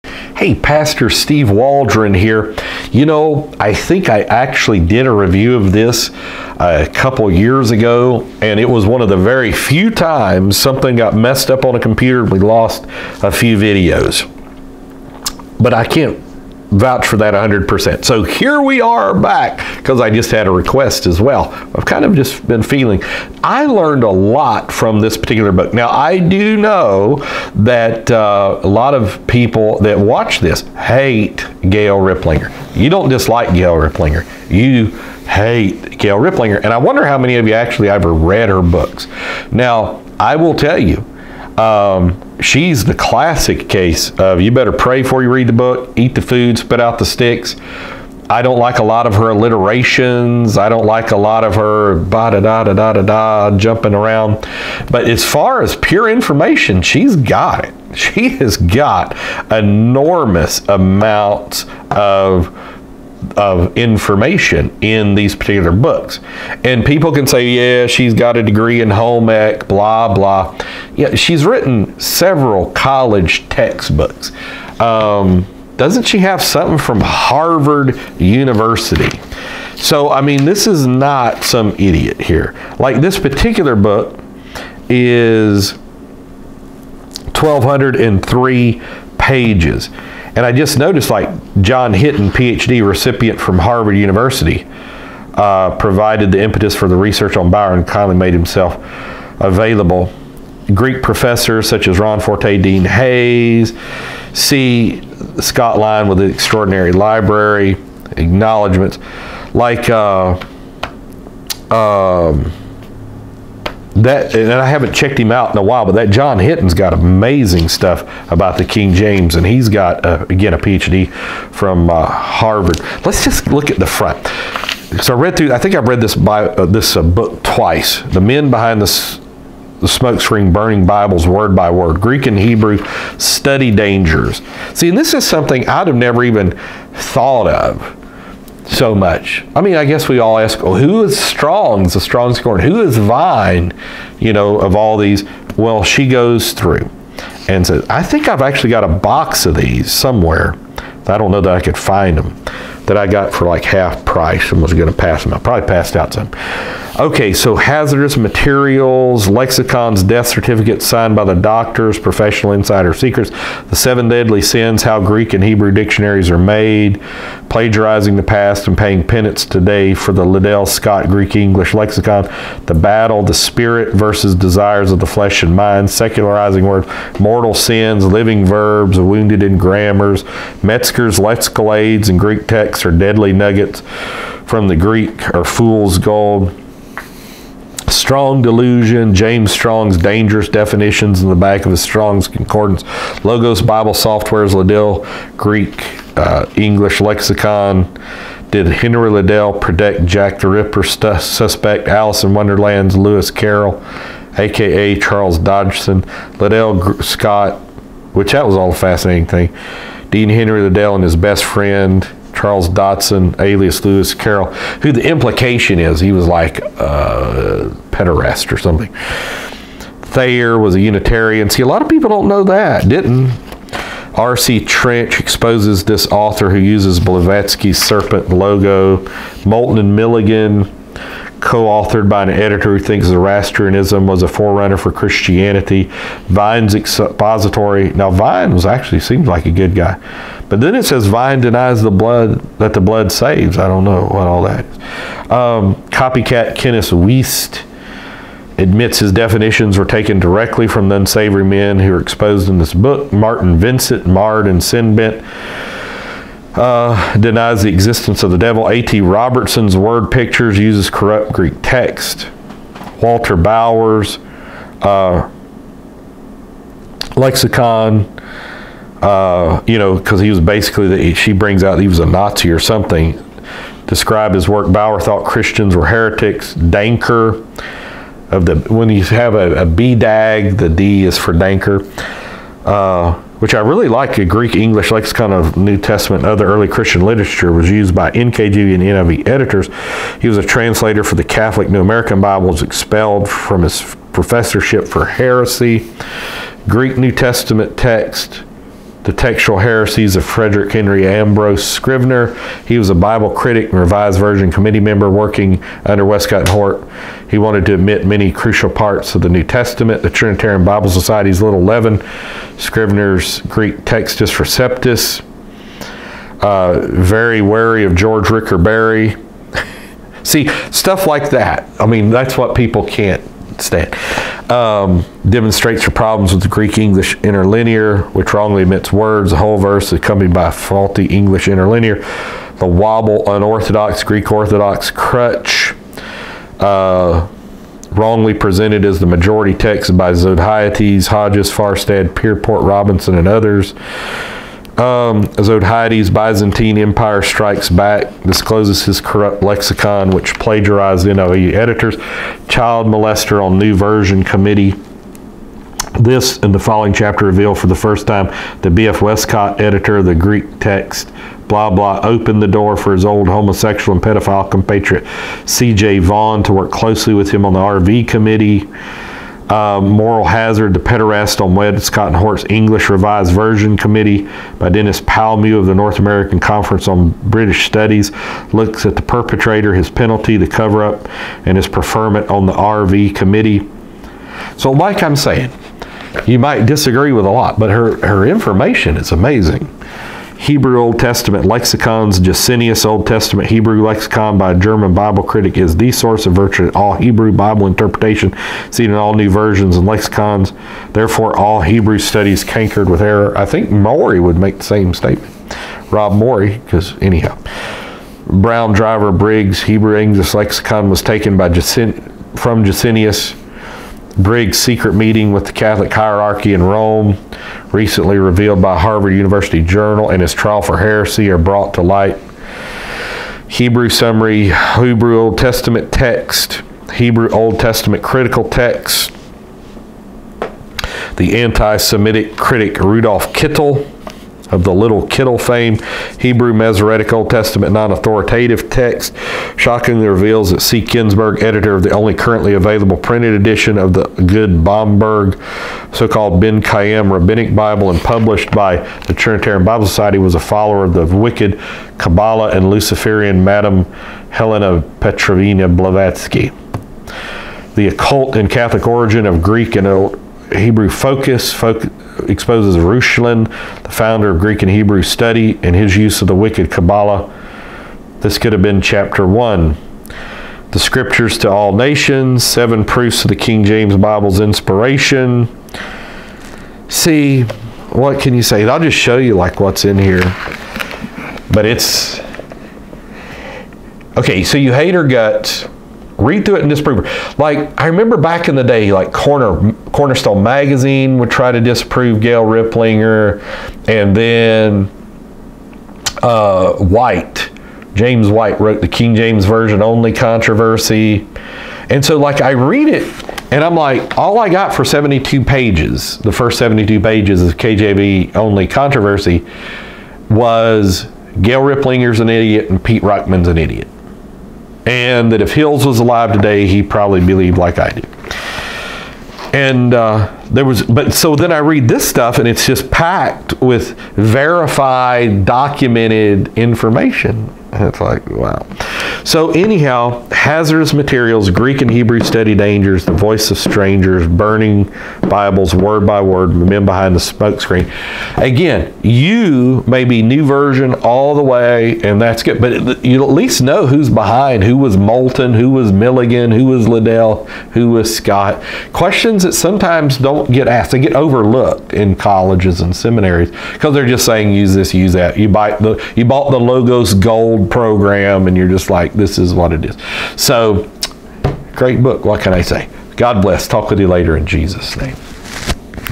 Review Hazardous Materials By Gail Riplinger
Gail Riplinger is a controversial but underrated scholar and researcher.  A look inside her voluminous work on Word Studies!